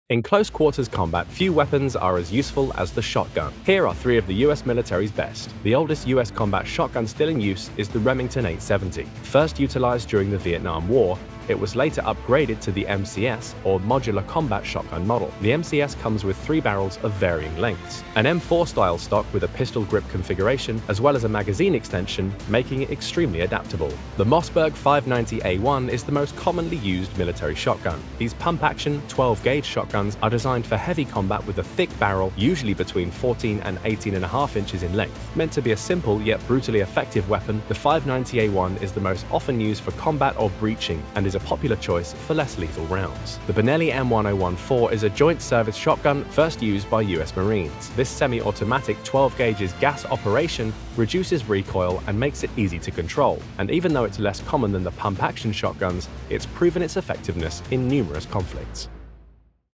Best Shotguns of the US Military
In close-quarters combat, few weapons are as useful as the shotgun. Here are three of the US Military’s best.

The oldest US combat shotgun still in use is the Remington 870. First utilized during the Vietnam War, it was later upgraded to the MCS or modular combat shotgun model. The MCS comes with three barrels of varying lengths, an M4-style stock with a pistol grip configuration, as well as a magazine extension, making it extremely adaptable.

The Mossberg 590A1 is the most commonly used military shotgun. These pump-action, 12-gauge shotguns are designed for heavy combat with a thick barrel, usually between 14 and 18.5 inches in length. Meant to be a simple yet brutally effective weapon, the 590A1 is most often used for combat or breaching, and is a popular choice for less lethal rounds.

The Benelli M1014 is a joint-service shotgun first used by US marines. This semi-automatic 12-gauge’s gas operation reduces recoil and makes it easy to control, and even though it’s less common than pump action shotguns, it’s proven its effectiveness in numerous conflicts